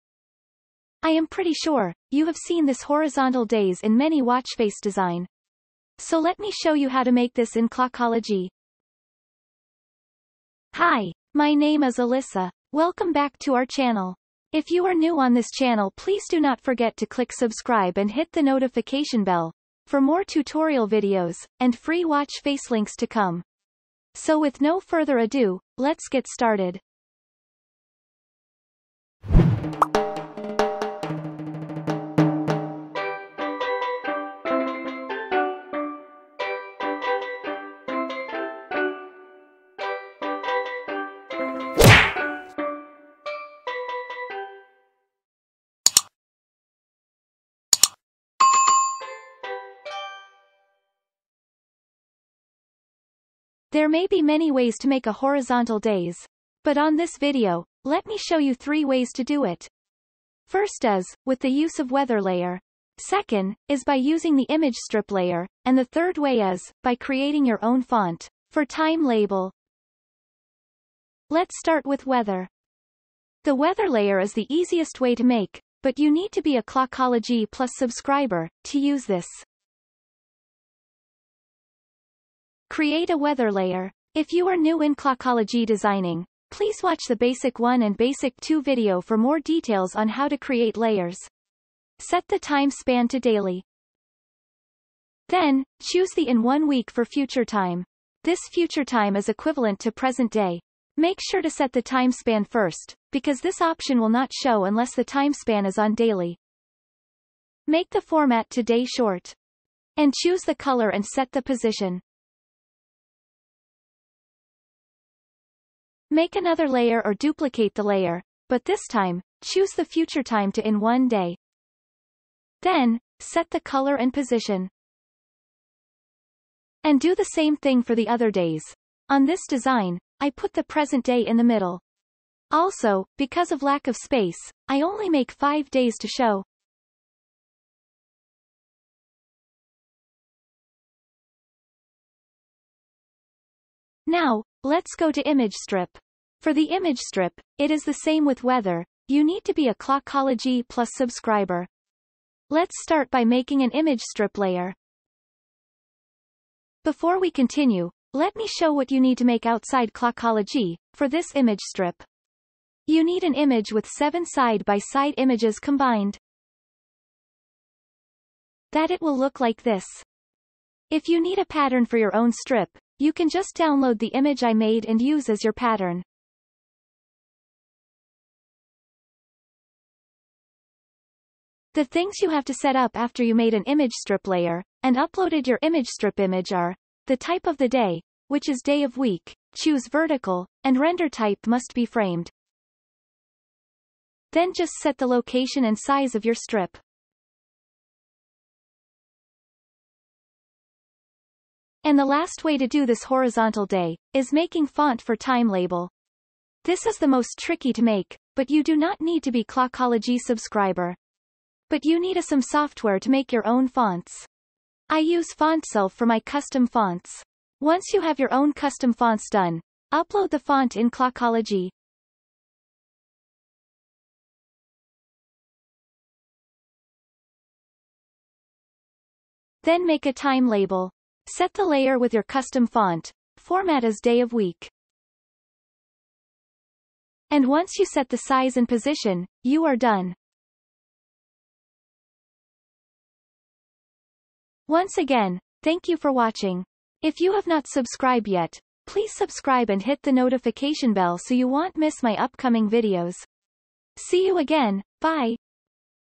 Clockology Tutorial: How to make a Horizontal Days in Clockology Apple watchface
This is the most basic way to design in Clockology apps.
A series of videos will be uploaded from Basic level up to Advance Level.

**Resources, materials, final watchface are provided for personal and educational purposes only.**

Final Watchface Link:
Multicolor (time label)

For some videos, resources needed will be provided for tutorial purposes.